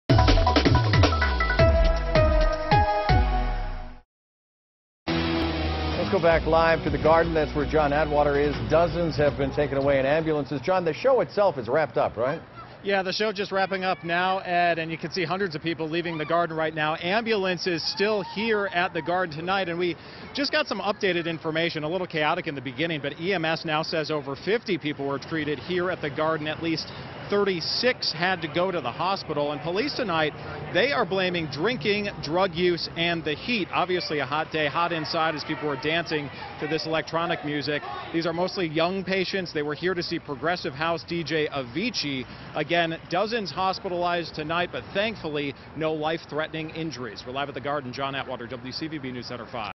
Drinking, drug use to blame for TD Garden incident
Officials said a combination of drugs, drinking and the heat are to blame for the mass casualty incident at TD Garden on Wednesday night.